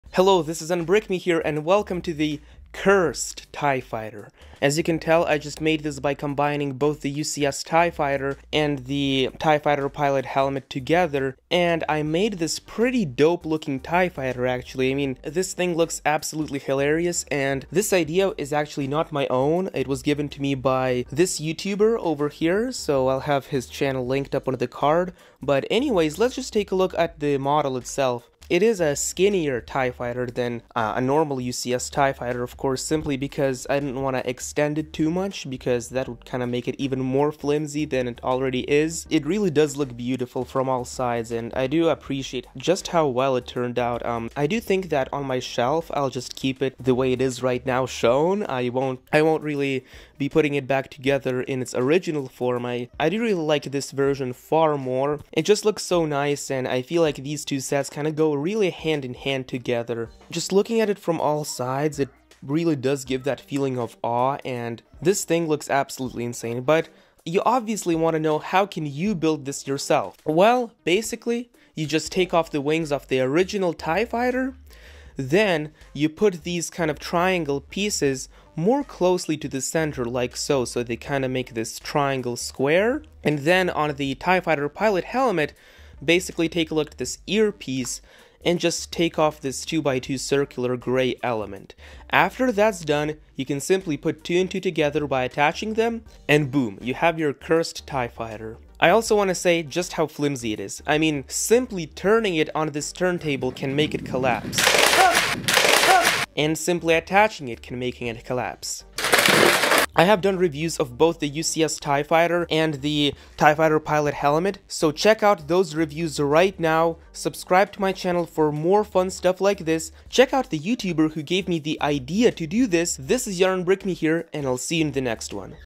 LEGO Cursed Tie Fighter (Star Wars 75095+75274)
Today, I decided to make a LEGO Star Wars Cursed Tie Fighter by combining the LEGO Star Wars Tie Fighter Pilot Helmet 75274 and the  LEGO Star Wars UCS TIE Fighter 75095. I really hope you enjoy this model. This idea was actually brought to light by DK Brickfilms, a very awesome LEGO YouTuber. You should go and send him some love.